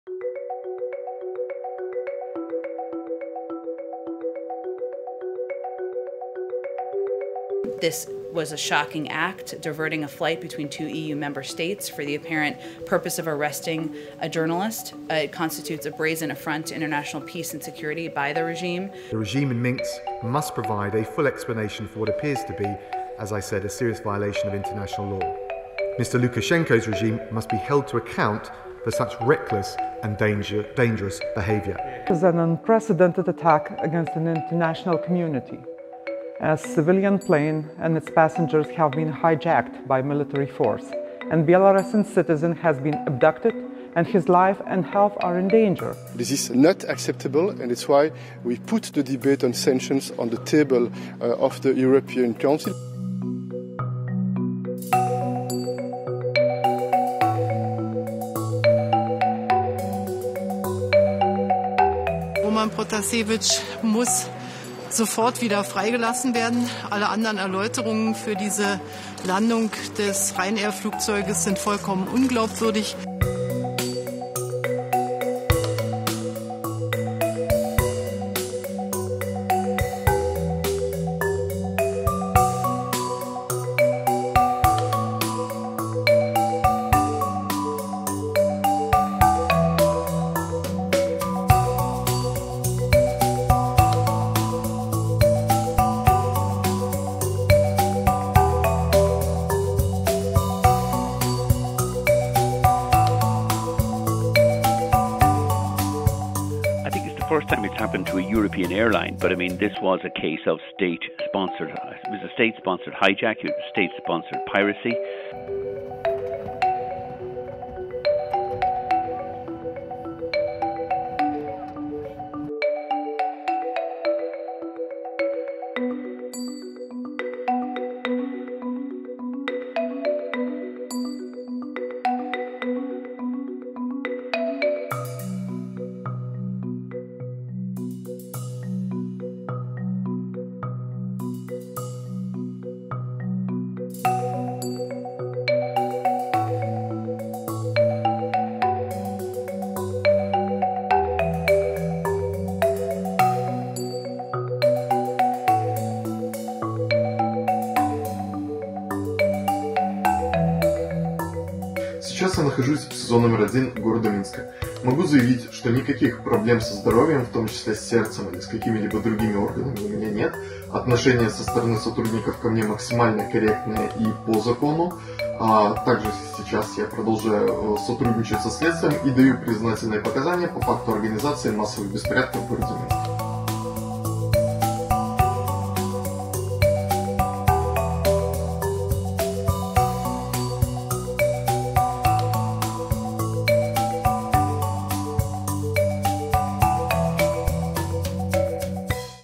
Belarus Journalist Arrested in Gov't Plane Hijack
‘This is state-sponsored hijack’ — Here’s why a rerouted Ryanair flight is causing Belarus’ government to face international backlash.